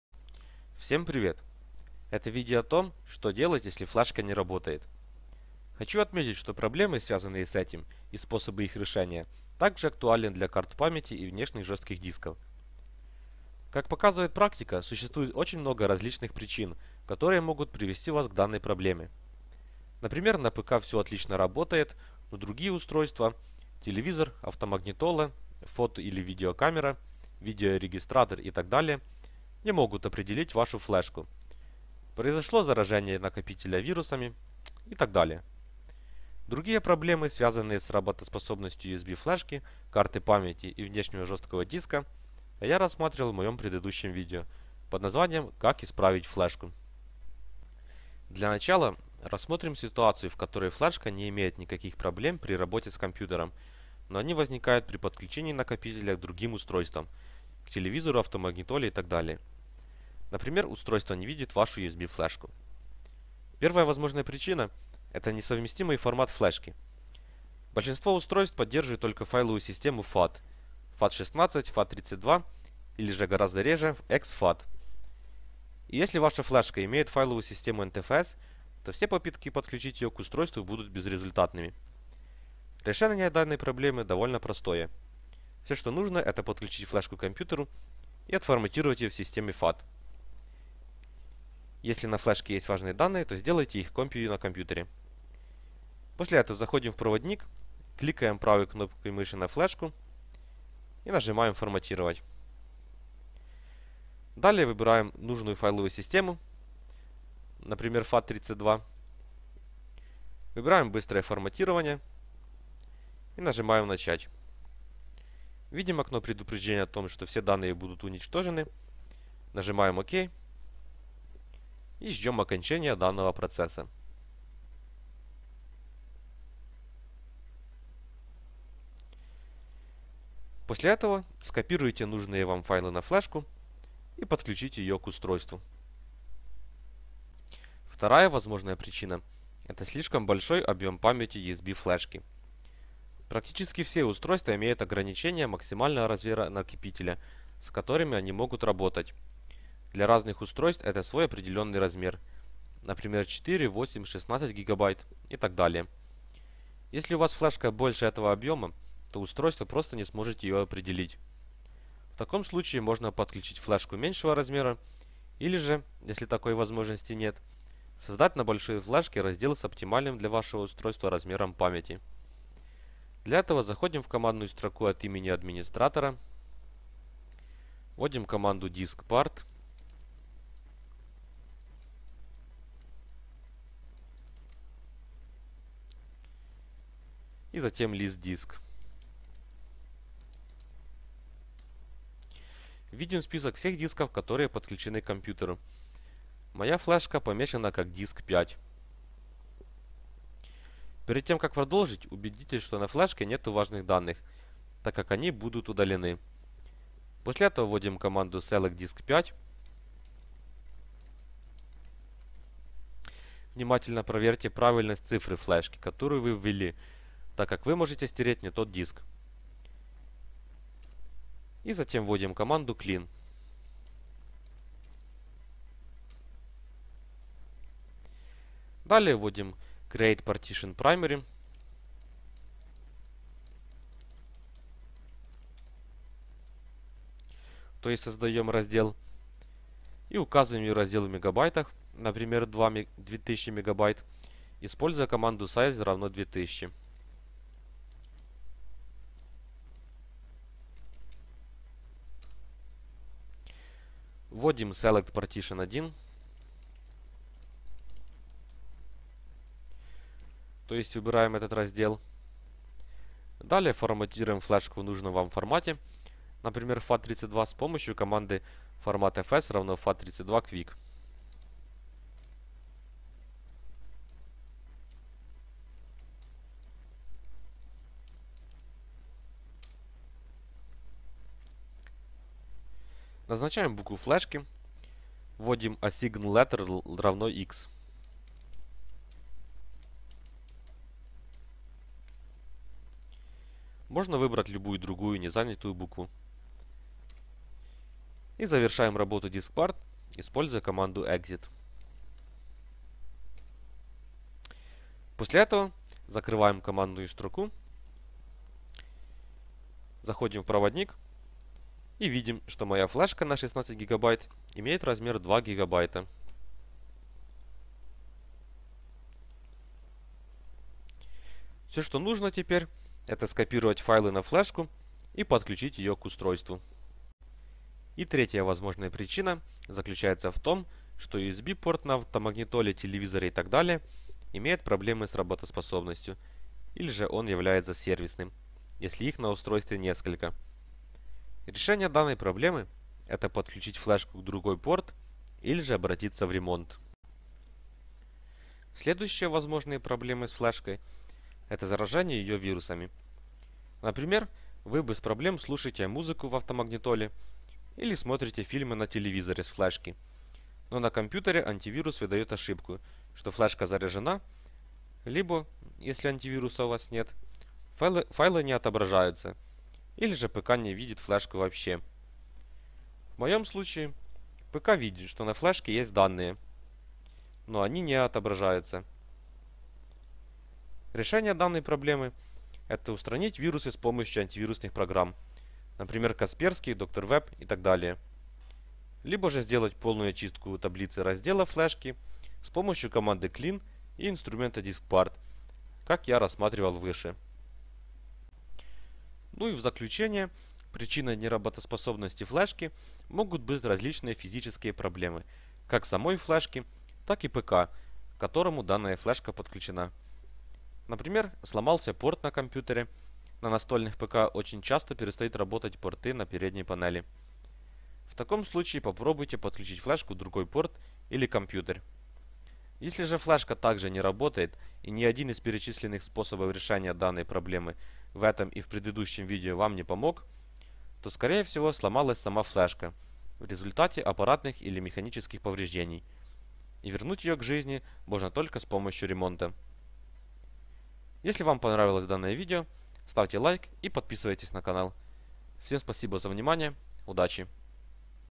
Флешка не работает с автомагнитолой, телевизором, TV приставкой, камерой, видеорегистратором 🛠️👨‍💻🤔
👍 Смотрите что делать, если флешка не работает. Как показывает практика, существует очень много различных причин, которые могут привести вас к данной проблеме. Например, на ПК все отлично работает, но другие устройства (телевизор, автомагнитола, фото- или видеокамера, видеорегистратор и т.д.) не могут определить вашу флешку, произошло заражение накопителя вирусами и т.д.

Читайте, USB флешка не читается – как восстановить данные?

Содержание:
00:00 - Вступление;
1:03 - Несовместимый формат флешки
2:17 - Слишком большой объем памяти USB флешки
5:27 - USB-порт имеет проблемы с работоспособностью или же он является сервисным

Другие проблемы, связанные с работоспособностью USB-флешки, карты памяти и внешнего жесткого диска я рассматривал в моем предыдущем видео под названием "Как исправить флешку".
Для начала, рассмотрим ситуацию, в которой флешка не имеет никаких проблем при работе с компьютером, но они возникают при подключении накопителя к другим устройствам (к телевизору, автомагнитоле и т.д.). Например, устройство не видит вашу USB-флешку.
Первая возможная причина – это несовместимый формат флешки. Большинство устройств поддерживают только файловую систему FAT (FAT16, FAT32 или же гораздо реже exFAT). И если ваша флешка имеет файловую систему NTFS, то все попытки подключить её к устройству будут безрезультатными. Решение данной проблемы довольно простое. Все что нужно, это подключить флешку к компьютеру и отформатировать её в системе FAT (если на флешке есть важные данные, то сделайте их копию на компьютере). После этого, заходим в проводник, кликаем правой кнопкой мыши на флешку и нажимаем "форматировать". Далее выбираем нужную файловую систему (например, FAT32), выбираем "быстрое форматирование" и нажимаем "начать". Видим окно предупреждения о том, что все данные будут уничтожены, нажимаем "Ок" и ждем окончания данного процесса. После этого, скопируйте нужные Вам файлы на флешку и подключите её к устройству.
Вторая возможная причина – это слишком большой объем памяти USB флешки. Практически все устройства имеют ограничения максимального размера накопителя, с которыми они могут работать (для разных устройств это свой определенный размер, например, 4, 8, 16 гб и т.д.). Если у вас флешка больше этого объема, то устройство просто не сможет её определить. В таком случае, можно подключить флешку меньшего размера или же, если такой возможности нет, создать на большой флешке раздел с оптимальным для вашего устройства размером памяти. Для этого, заходим в командную строку от имени администратора, вводим команду "Diskpart" и затем "list disk". Видим список всех дисков, которые подключены к компьютеру. Моя флешка помечена как диск 5. Перед тем как продолжить, убедитесь, что на флешке нету важных данных, так как они будут удалены. После этого, вводим команду "select disk 5" (внимательно проверьте правильность цифры флешки, которую вы ввели, так как Вы можете стереть не тот диск) и затем вводим команду "clean". Далее вводим "create partition primary" (то есть создаем раздел) и указываем его размер в мегабайтах (например, 2000 Мб) используя команду "size=2000". Вводим "select partition 1" (выбираем этот раздел), далее форматируем флешку в нужном Вам формате (например, в FAT32) с помощью команды "format fs=fat32 quick", назначаем букву флешки – вводим "assign letter=X" (можно выбрать любую другую не занятую букву) и завершаем работу "Diskpart" используя команду "exit". После этого, закрываем командную строку и видим, что моя флешка на 16 Гб имеет размер 2 Гб. Все, что нужно теперь – это скопировать файлы на флешку и подключить её к устройству.
И третья возможная причина заключается в том, что USB-порт на автомагнитоле, телевизоре и т.д. имеет проблемы с работоспособностью или же он является сервисным (если их на устройстве несколько). Решение данной проблемы – это подключить флешку в другой порт или же обратится в ремонт.
Следующие возможные проблемы с флешкой – это заражение её вирусами. Например, вы без проблем слушаете музыку в автомагнитоле или смотрите фильмы на телевизоре с флешки, но на компьютере антивирус выдает ошибку, что флешка заражена либо, если антивируса у Вас нет, файлы не отображаются или же ПК не видит флешку вообще. В моем случае, ПК видит, что на флешке есть данные, но они не отображаются. Решение данной проблемы – это устранить вирусы с помощью антивирусных программ (например, Kaspersky, DrWeb и так далее), либо же сделать полную очистку таблицы раздела флешки с помощью команды clean и инструмента Diskpart, как я рассматривал выше.

Смотрите другие ролики на тему: НеРаботаетФлешка, Автомагнитола, Телевизор, Камера, ВидеоПриставка, ВидеоРегистратор.